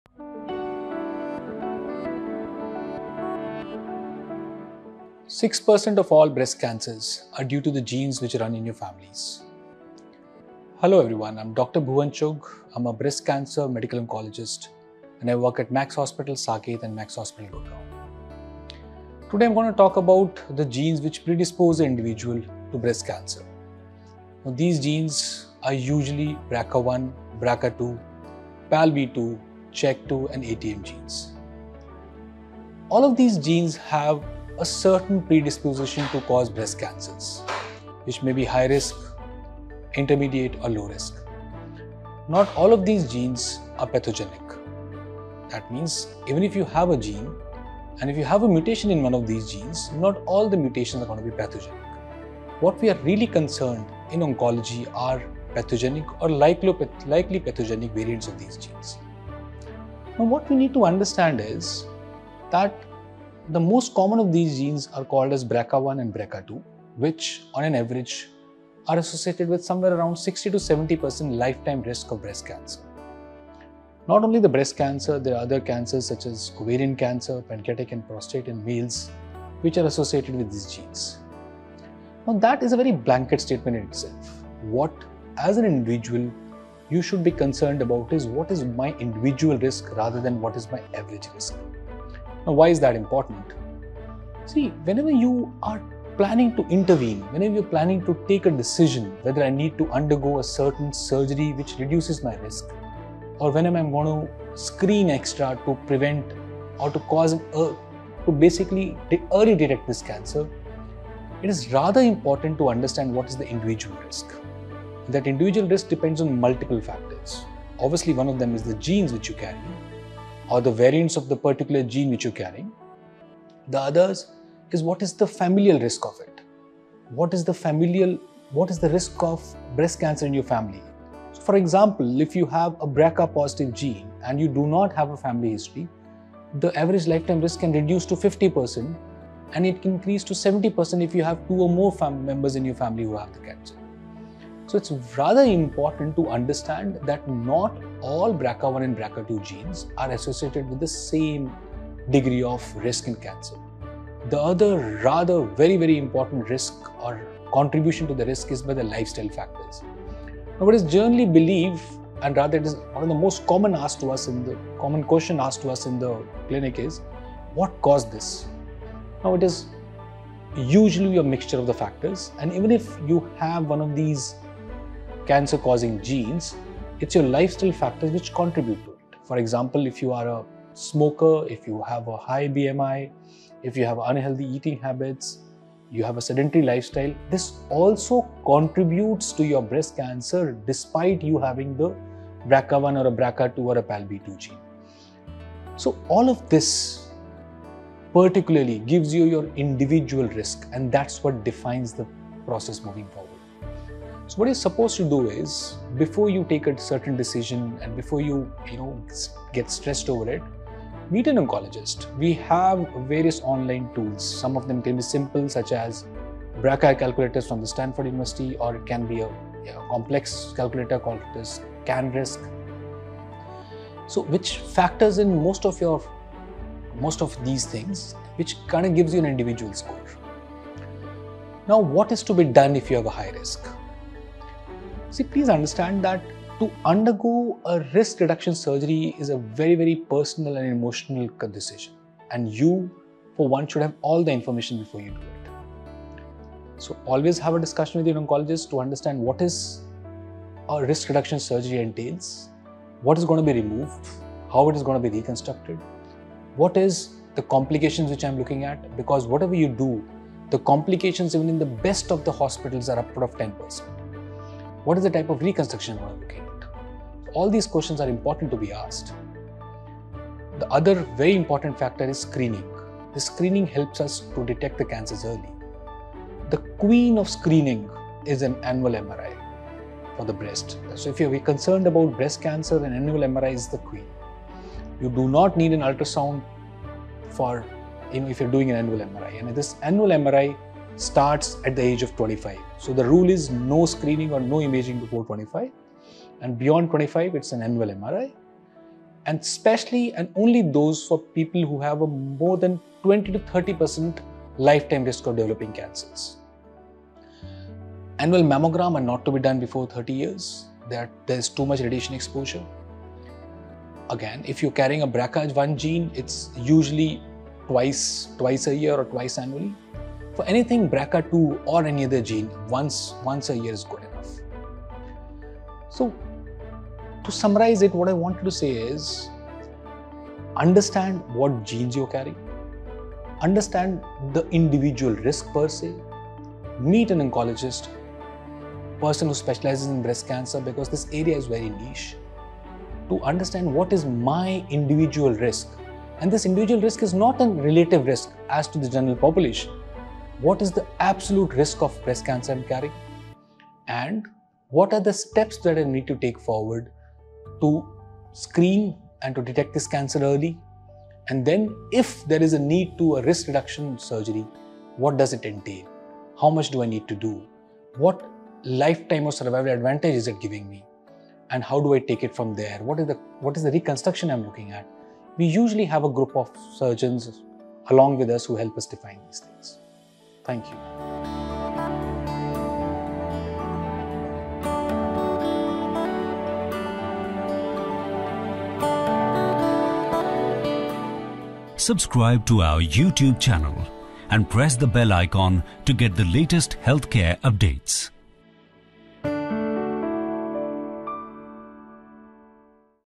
Role of BRCA Gene Mutation | Dr. Bhuvan Chugh | Max Hospital, Gurgoan
Wondering how BRCA gene mutations impact breast cancer risk? Join Dr. Bhuvan Chugh, Senior Consultant, Medical Oncology, Max Hospital, Gurgoan, as he explains these critical genes, their effects on your health, and how to take proactive steps. This video offers clear insights into risk factors, screening methods, and prevention strategies. Watch now to get the knowledge you need to stay ahead of your health.